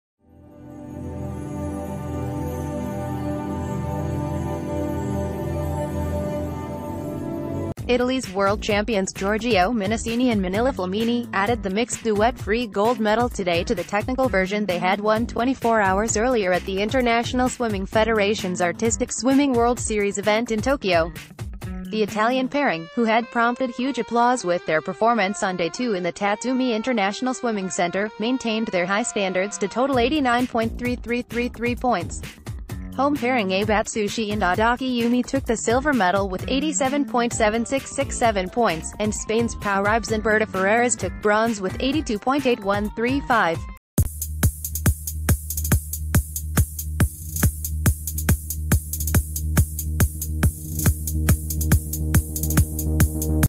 Breaking News | Italian world champions win second gold at FINA Artistic Swimming World Series in T
Breaking News | Italian world champions win second gold at FINA Artistic Swimming World Series in Tokyo

Italy’s world champions Giorgio Minisini and Manila Flamini, added the mixed duet free gold medal today to the technical version they had won 24 hours earlier at the International Swimming Federation’s Artistic Swimming World Series event in Tokyo. The Italian pairing, who had prompted huge applause with their performance on day two in the Tatsumi International Swimming Centre, maintained their high standards to total 89.3333 points. Home pairing Abe Atsushi and Adachi Yumi took the silver medal...